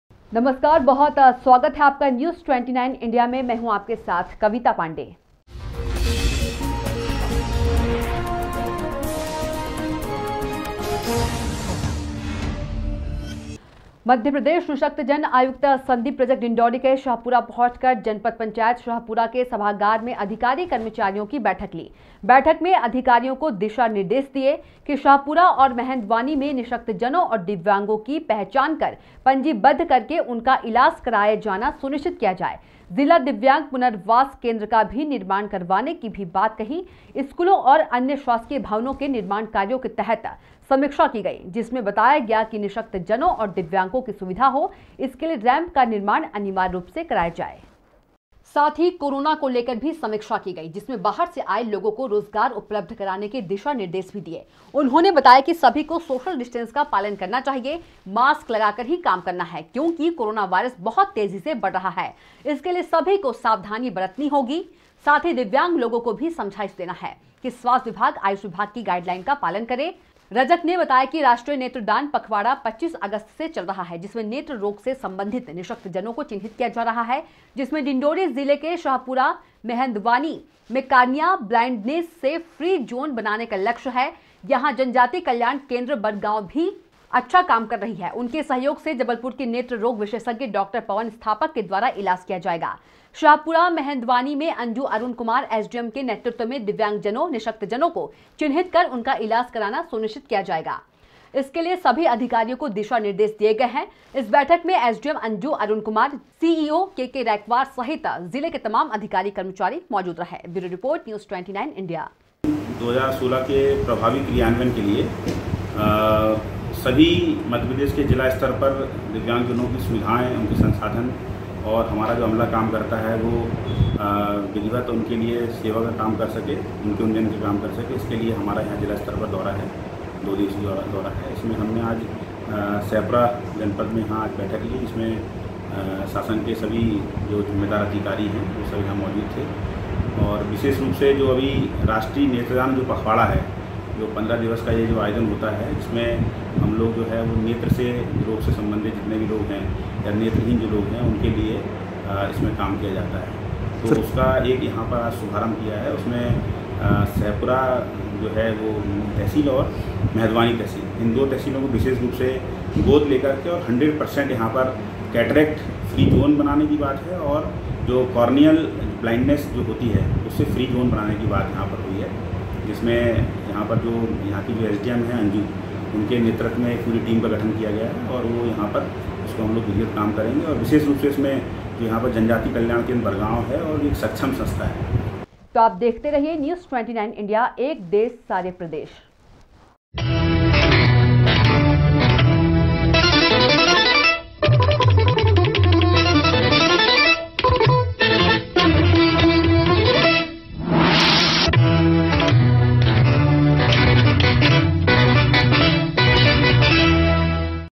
News29India#News 08#शहपुरा के सभागार में अधिकारी-कर्मचारियों की ली बैठक
शहपुरा के सभागार में अधिकारी-कर्मचारियों की ली बैठक
दिव्यांग पुनर्वास केन्द्र का भी निर्माण करवाने की भी बात कही